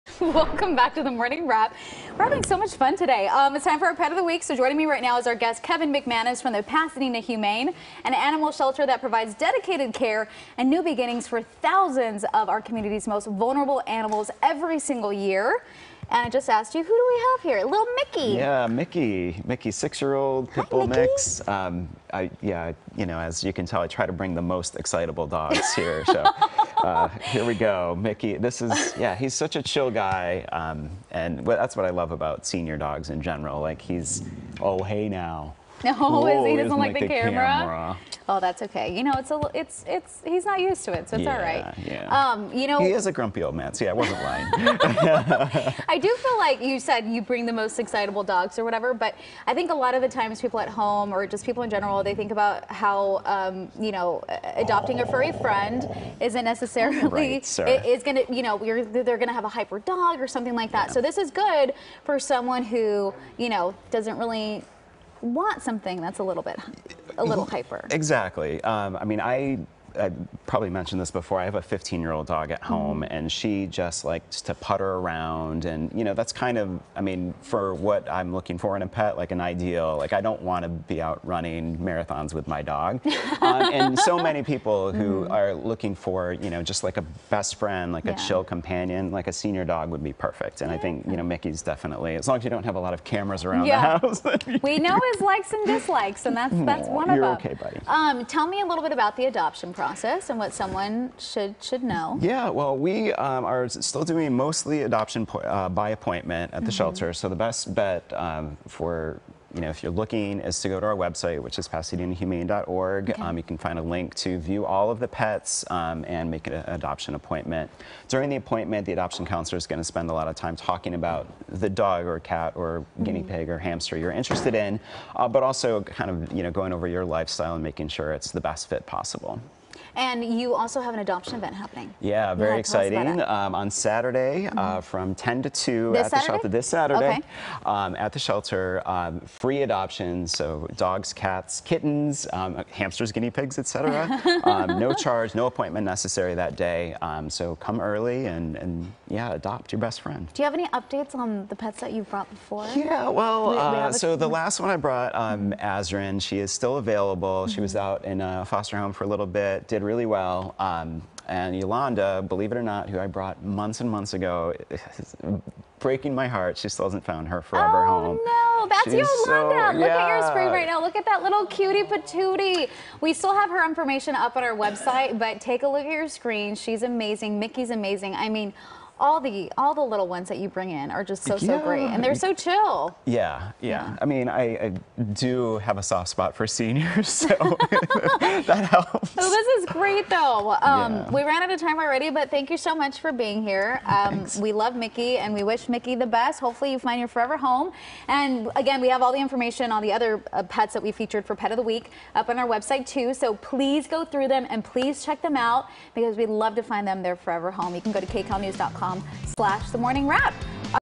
Pet of the Week: Micky
Micky is a six-year-old pit bull mix looking for a forever home: Pasadena Humane